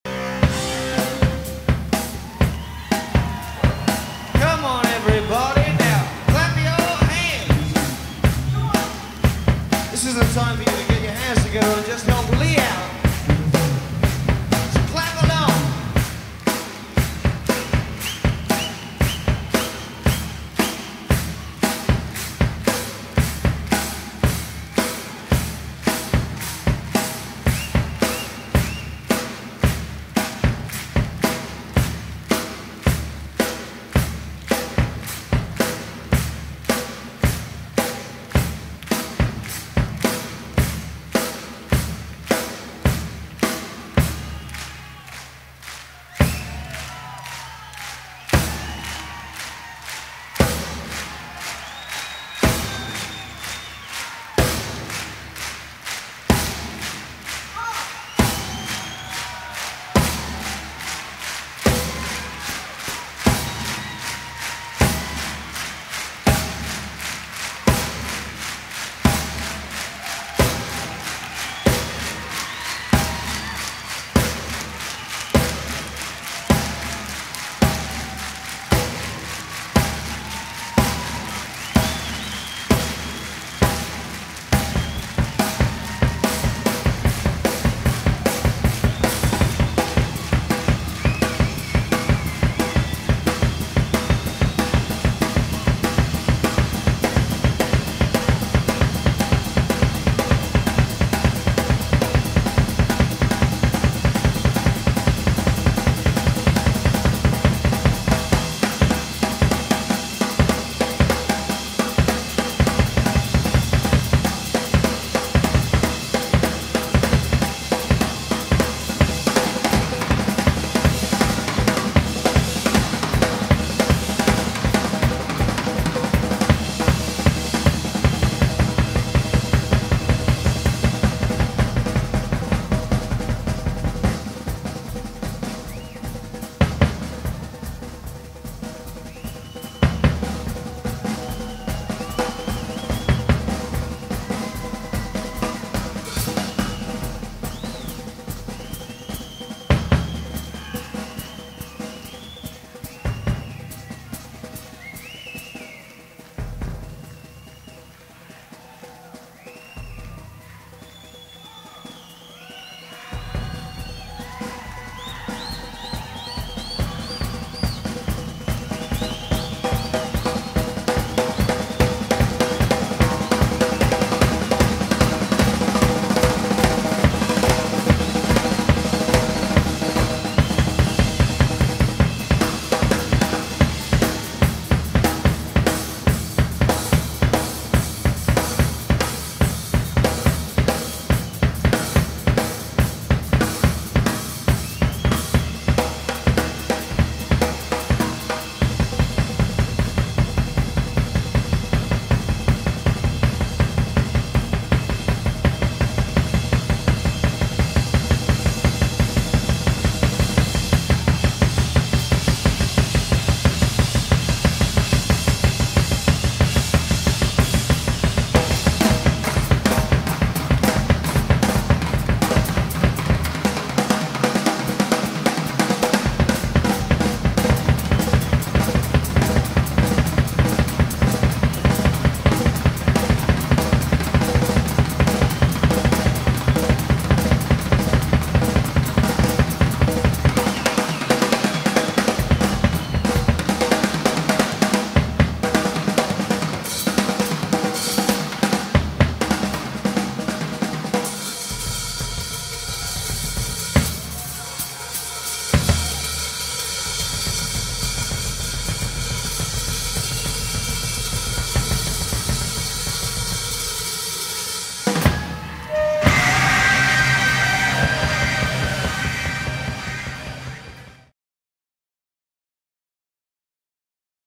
Lee Kerslake drum solo c.1974 (R.I.P.)
Lee is among the greatest drummers in rock, on par with John Bonham, Keith Moon, Ginger Baker, Ian Paice, Bill Ward etc..his work with Uriah Heep and Ozzy Osbourne is one of the greatest rock records of all time and he has inspired so many aspiring musicians including myself.

Audio taken from Feb. 1974, San Diego CA

16 April 1947 – 19 September 2020